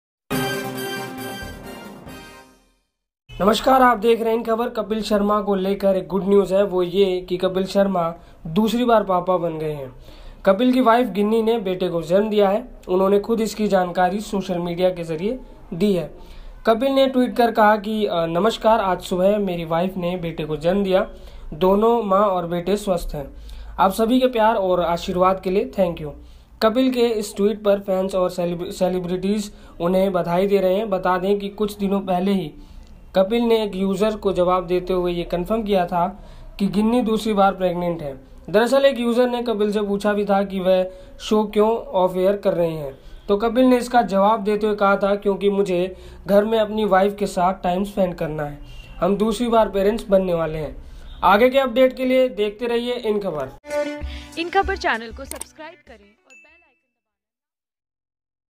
Actor-Comedian Kapil Sharma and Ginni Chatrath blessed with baby boy, दूसरी बार पिता बने कपिल
अपनी शानदार कॉमेडी से लोगों का दिल जीतने वाले कपिल शर्मा 3 साल में दूसरी बार पिता बने हैं. कपिल की पत्नी गिन्नी चतरथ ने एक बेटे को जन्म दिया है जिसकी जानकारी कपिल शर्मा ने ट्वीट करके दी. हालांकि इस गूड न्यूज पर वो ट्रोल भी हो रहे हैं.